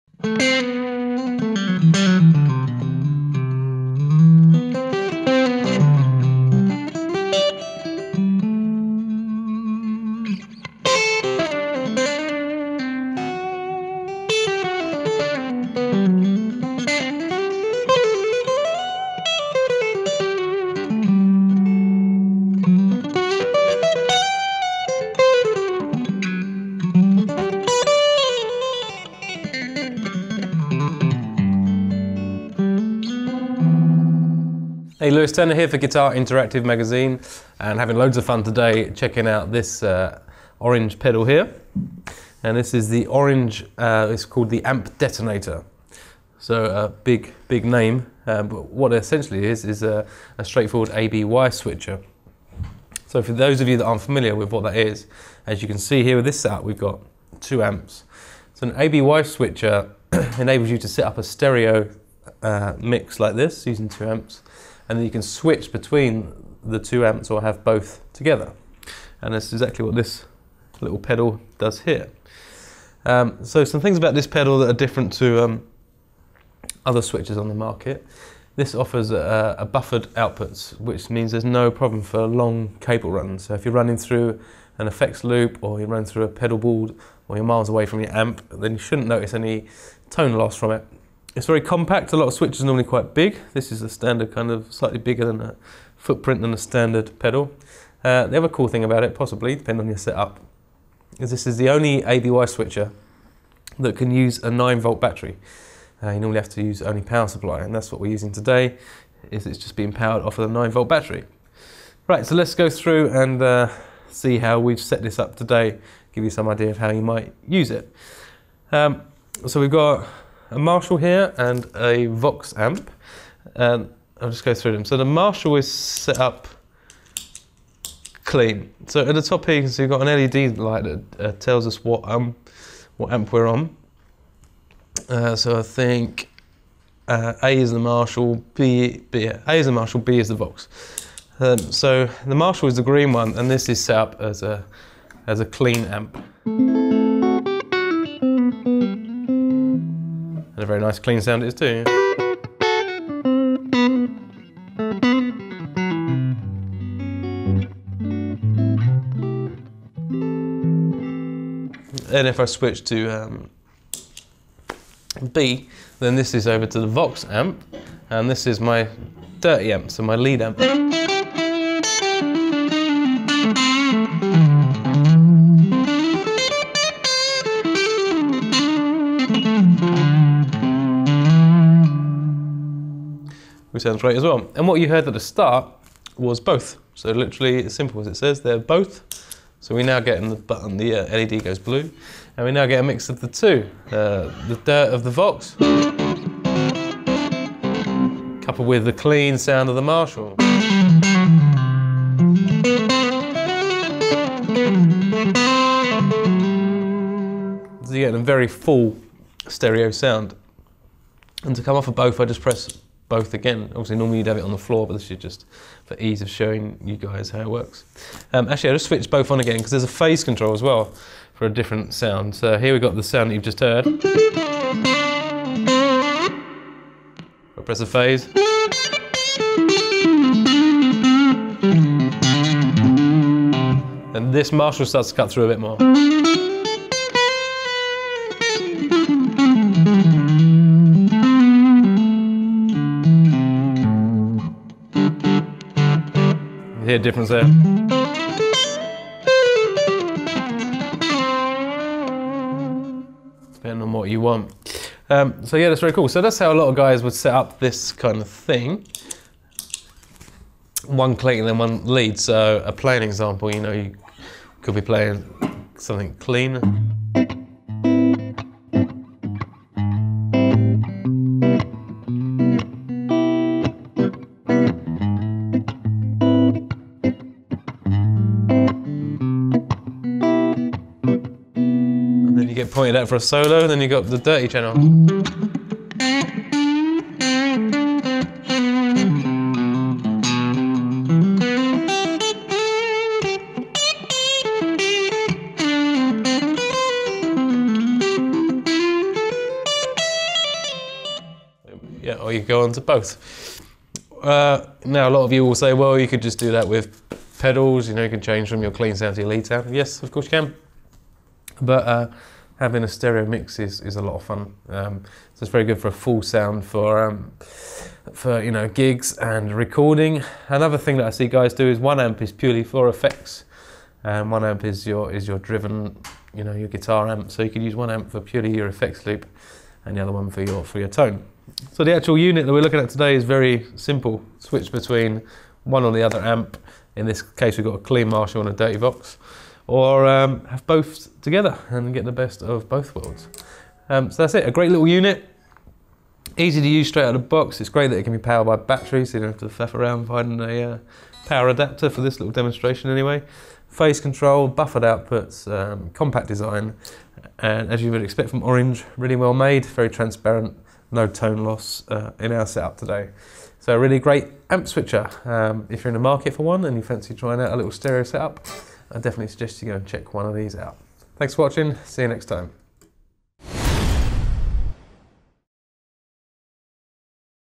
Orange Amp Detonator Pedal Review
From the makers of the Dual Terror, Crush and Rockerverb; Orange Music Electronic Company brings you the Orange Amp Detonator Pedal. This is an A/B/Y pedal that allows you to switch between two amps and blend them together. Lewis turner takes a closer look at this new addition to the Orange pedal line up.